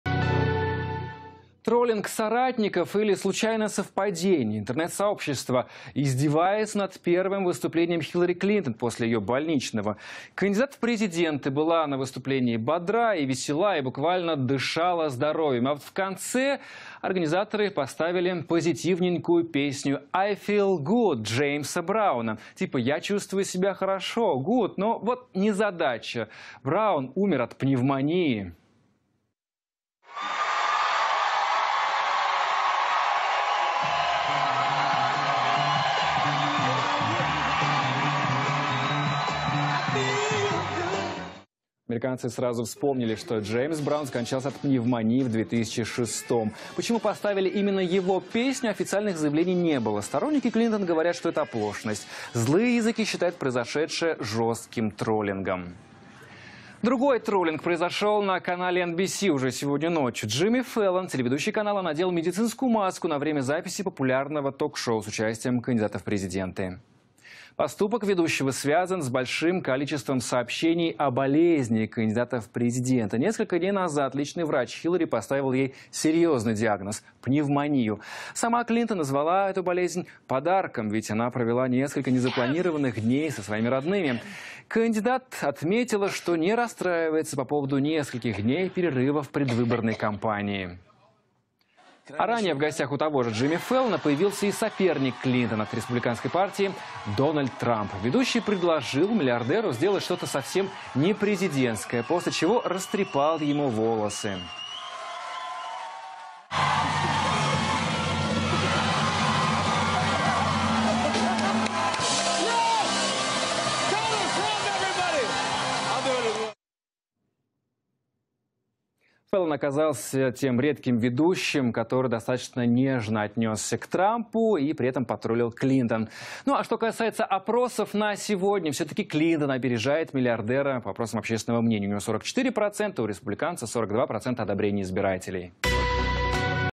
Клинтон процитировала умершего от пневмонии певца
Интернет-сообщество бурно отреагировало на иронию